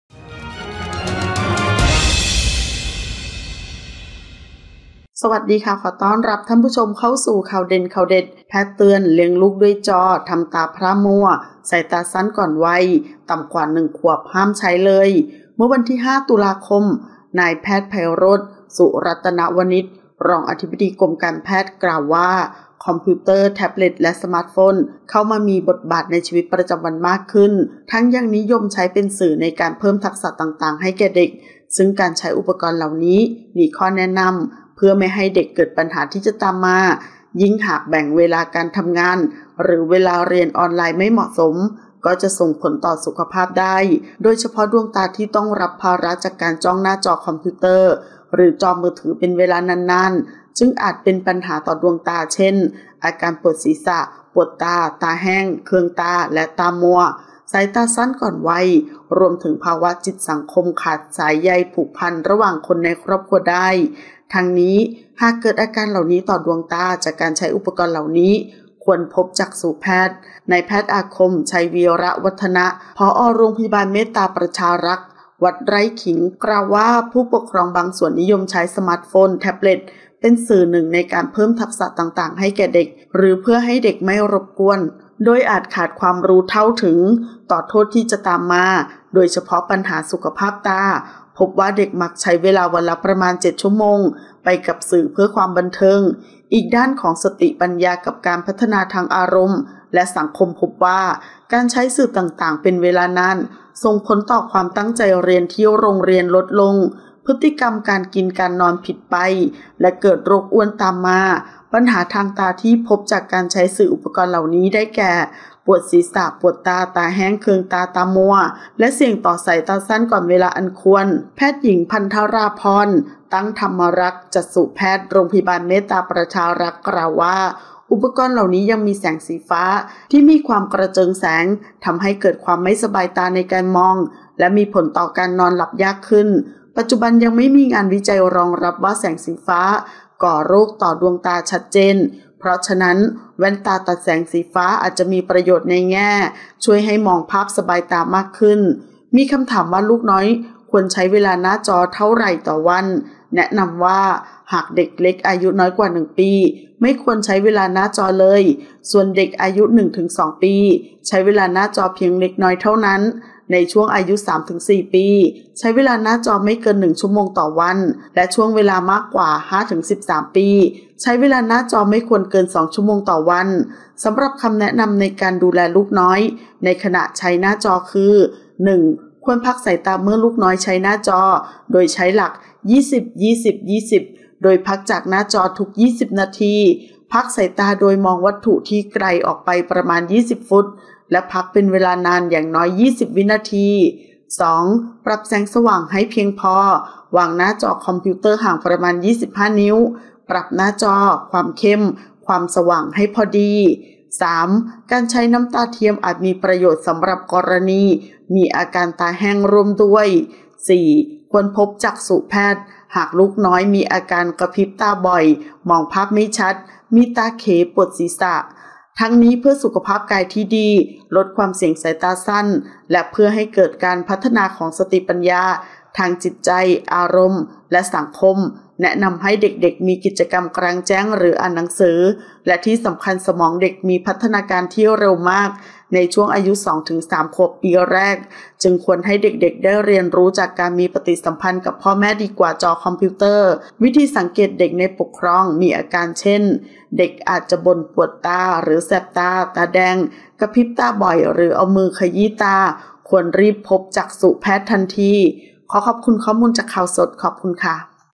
แพทย์เตือน เลี้ยงลูกด้วยจอ ทำตาพร่ามัว-สายตาสั้นก่อนวัย ต่ำกว่า 1 ขวบห้ามใช้เลย
00:00 - Intro
00:10 - News
01:00 - Minute